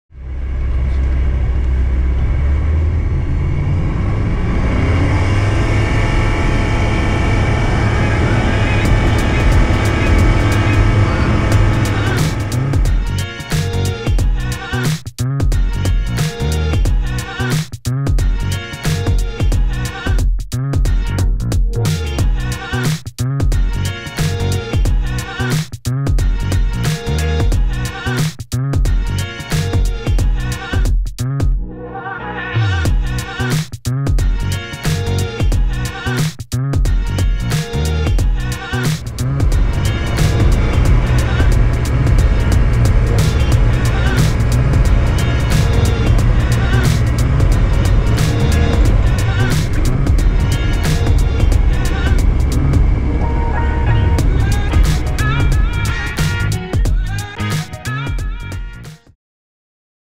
Flying in 20 kt Cross Winds in a Bristell
Another incredible video by Midland Media displaying the capabilities of the Bristell